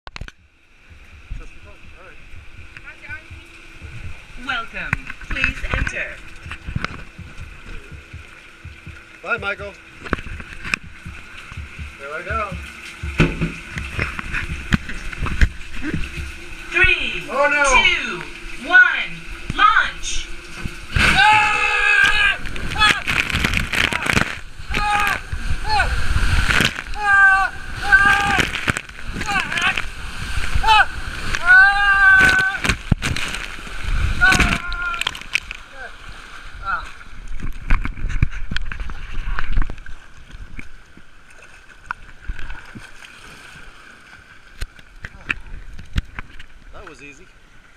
Dad on Zero G TOTAL FREAKOUT!!! @ Action Park
Dad goes on Zero G trapdoor waterslide and Freaks out for the entire time. FUNNY!